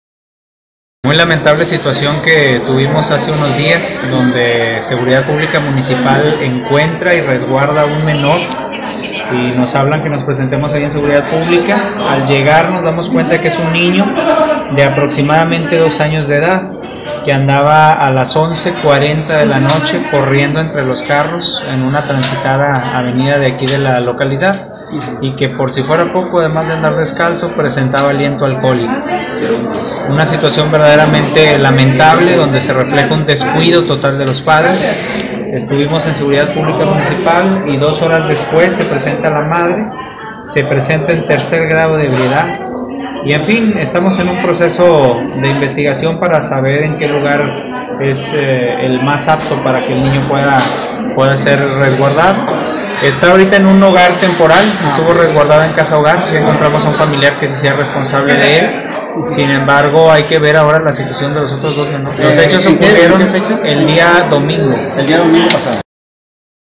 ENCUENTRAN A MENOR DEAMBULANDO EN LA CALLE; PRESENTABA ALIENTO ALCOHÓLICO
Eliud Cadena Toledo director de la Procuraduría del niño, la niña y la familia (Pronnif) informó que el pasado domingo se registró un incidente donde un menor de 2 años fue encontrado deambulando por la calle a las 11:40 de la noche en una avenida principal de la ciudad.
Por si fuera poco, el menor se encontraba descalzo y presentaba aliento alcohólico.
Ante este problema, el menor fue resguardado por Seguridad Pública y de ahí se le hizo la notificación a la Pronnif para informar de la situación y contactar a los padres del menor.